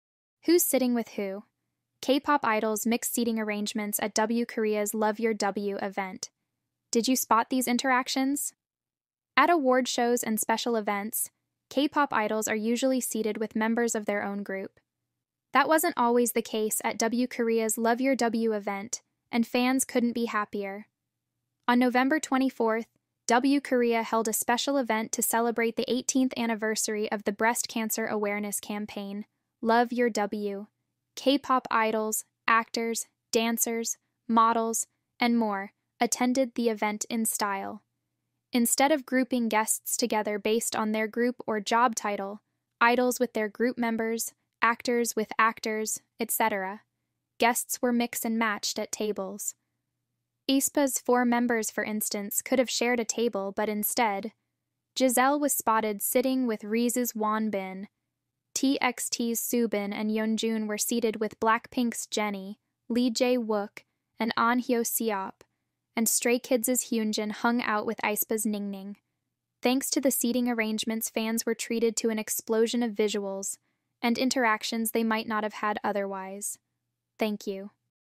Who’s Sitting With Who? K-Pop Idols’ Mixed Seating Arrangements At W Korea’s “Love Your W” Event
“Did you spot these interactions?”

At award shows and special events, K-Pop idols are usually seated with members of their own group. That wasn’t always the case at W Korea’s “Love your W” event — and fans couldn’t be happier!

On November 24, W Korea held a special event to celebrate the 18th anniversary of the breast cancer awareness campaign, “Love your W.” K-Pop idols, actors, dancers, models, and more attended the event in style.

Instead of grouping guests together based on their group or job title (idols with their group members, actors with actors, etc), guests were mix and matched at tables. aespa‘s four members, for instance, could have shared a table, but instead…

…Giselle was spotted sitting with RIIZE‘s Wonbin…

…TXT‘s Soobin and Yeonjun were seated with BLACKPINK‘s Jennie, Lee Jae Wook, and Ahn Hyo Seop…

…and Stray Kids‘s Hyunjin hung out with aespa’s Ningning.

Thanks to the seating arrangements, fans were treated to an explosion of visuals…

…and interactions they might not have had otherwise.